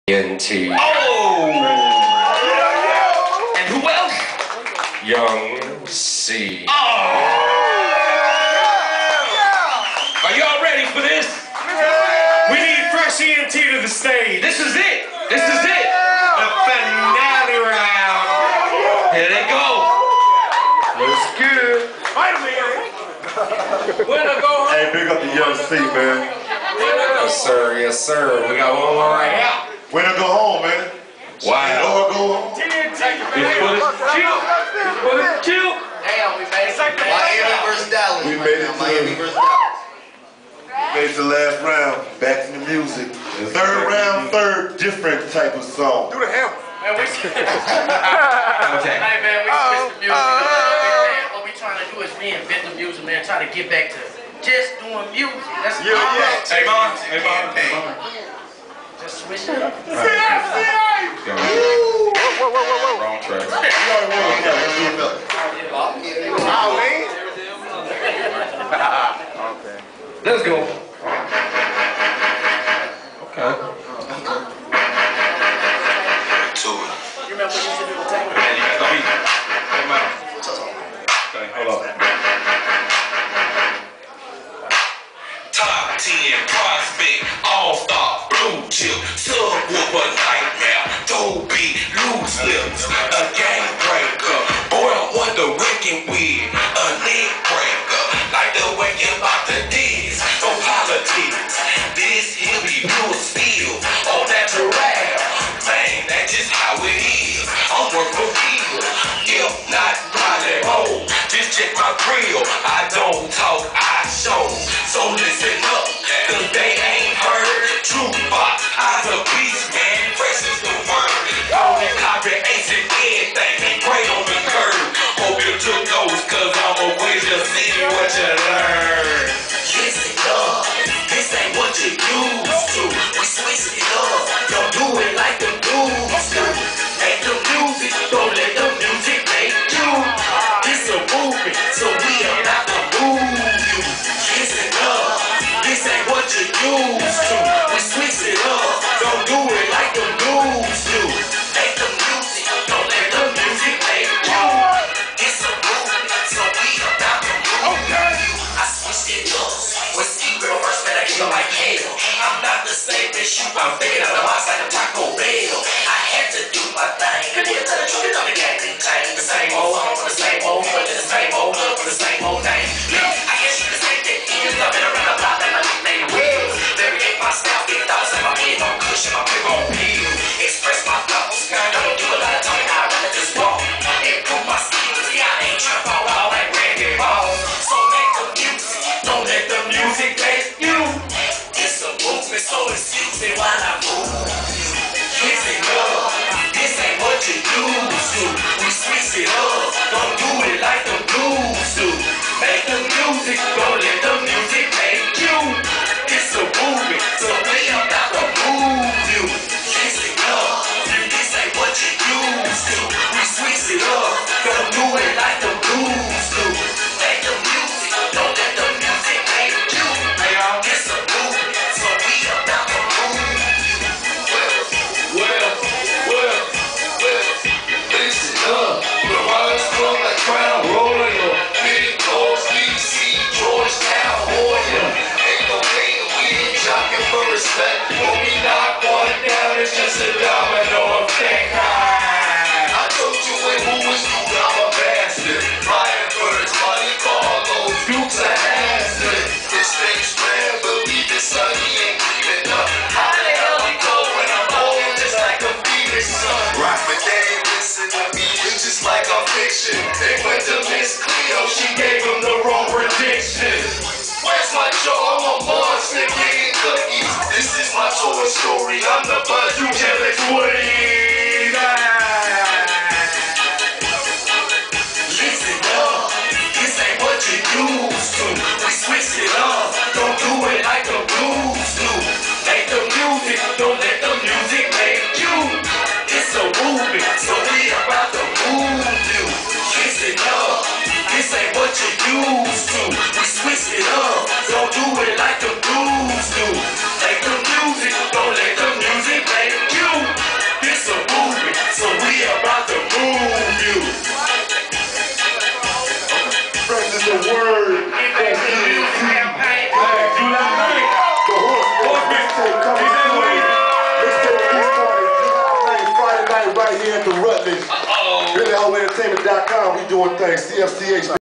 CHAMPIONSHIP ROUND.....UMC 3.2!!
FRESH ENT MADE IT TO THE FINAL ROUND...BEAT OUT SOME GREAT COMPETITION IN 1ST AND 2ND ROUND BUT FELL 1 POINT SHORT OF WINNING THE WHOLE THING...ITS ALL GOOD THOUGH...WE'LL BE BACK...NO MISTAKING...'FRESH IS DA WORD'!!!